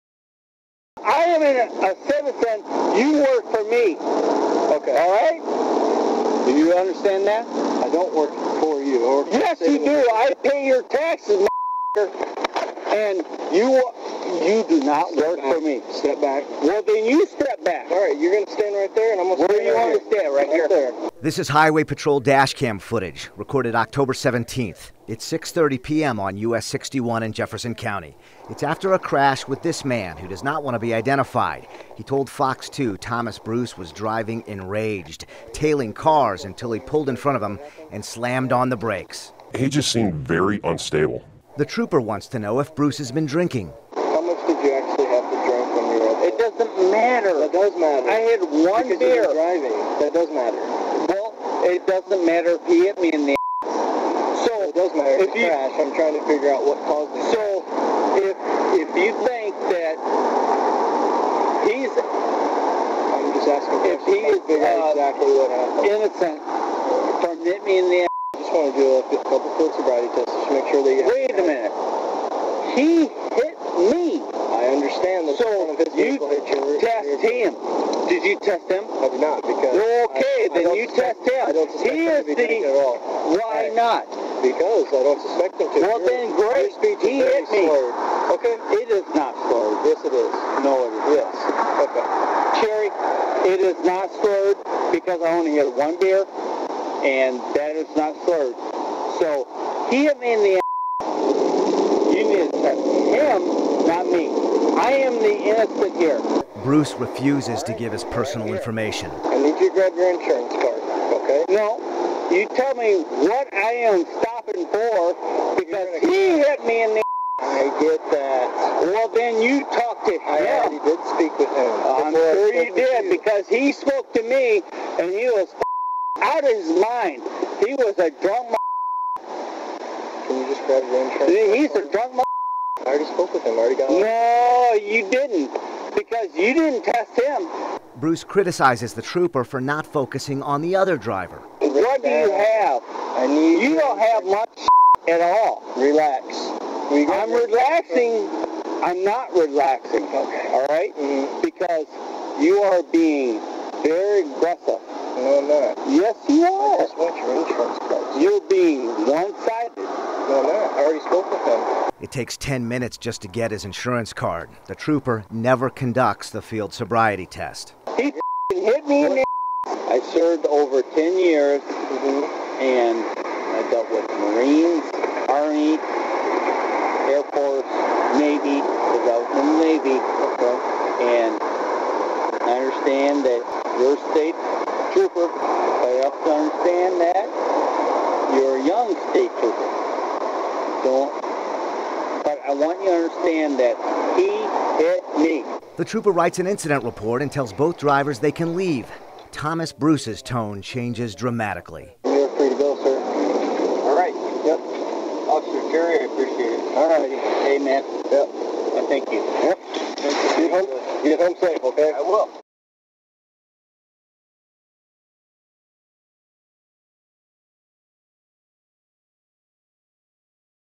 Murder suspect Thomas Bruce on Highway Patrol dashcam
Here is murder suspect Thomas Bruce ordering around a Highway Patrolman after a reported road rage incident October 17, 2018.  This was one month before his alleged Catholic store murder.